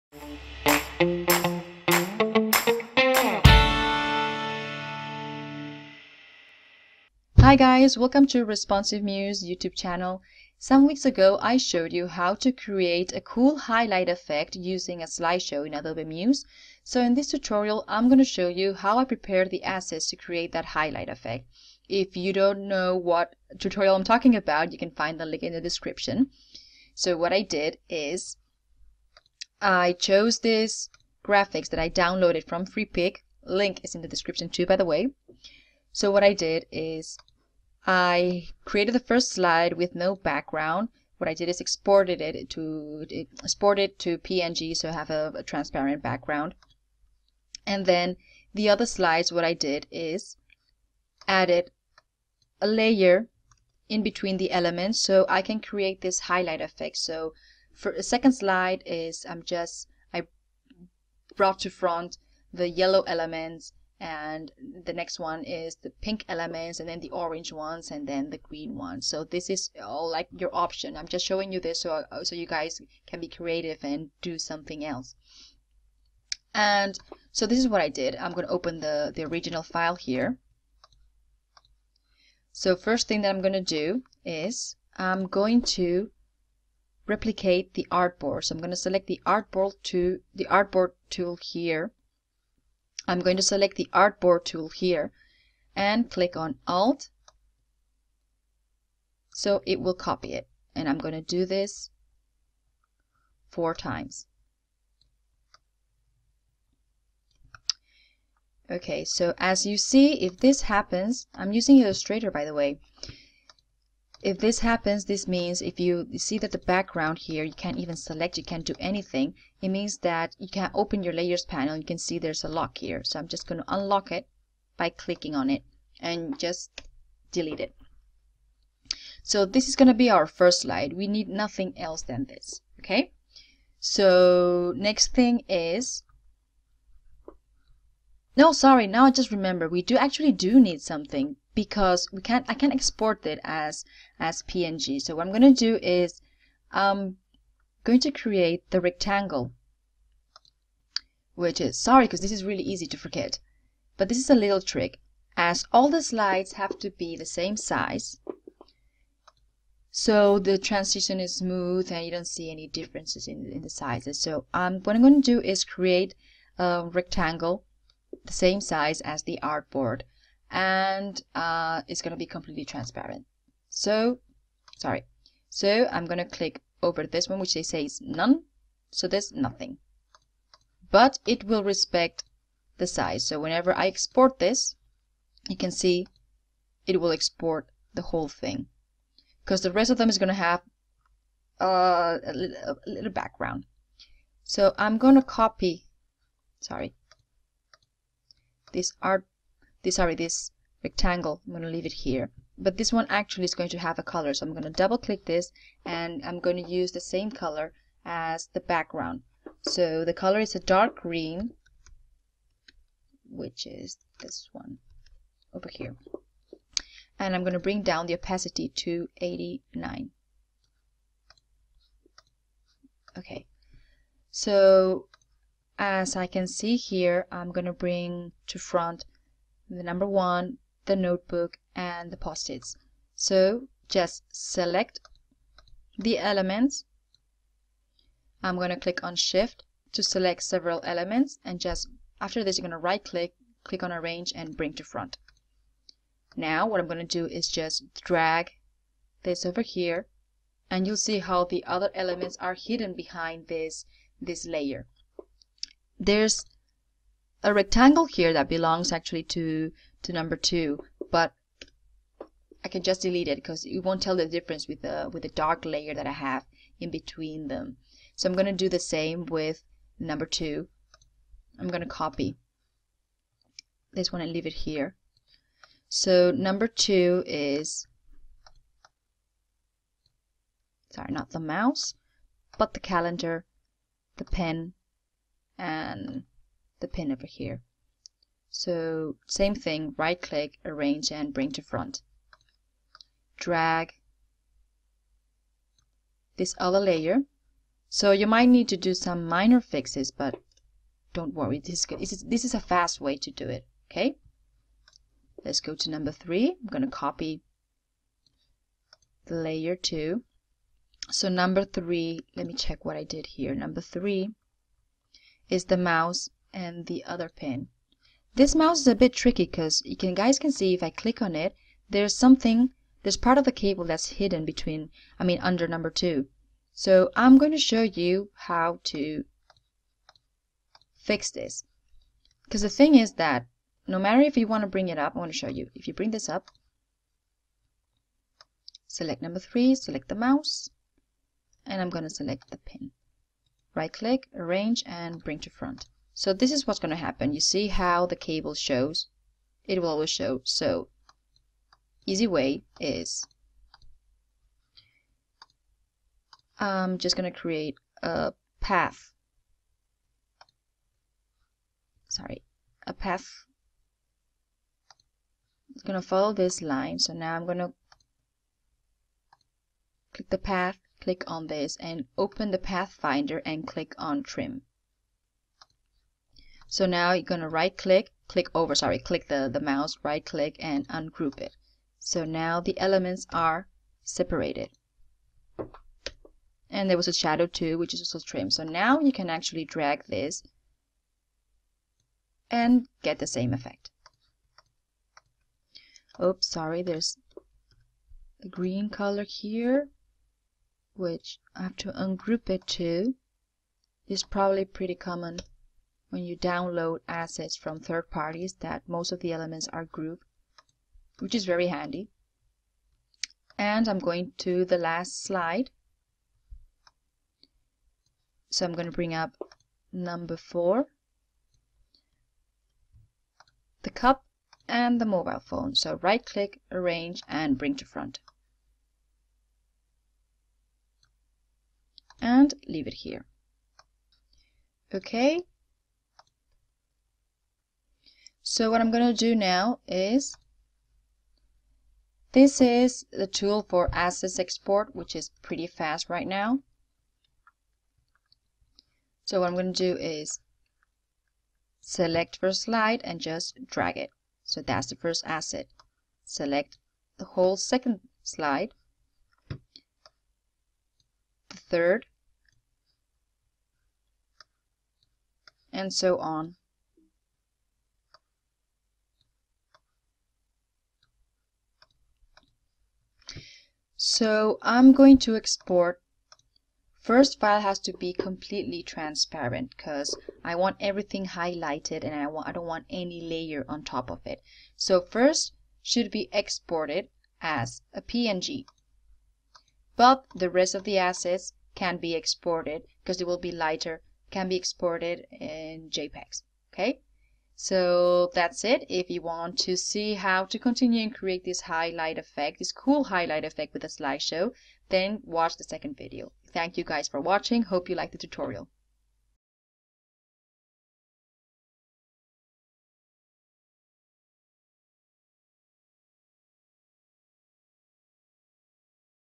Get your assets ready for Adobe Muse
Learn how to get your assets ready to use them to create a cool Highlight effect using a slideshow in Adobe Muse. No third party widgets are required!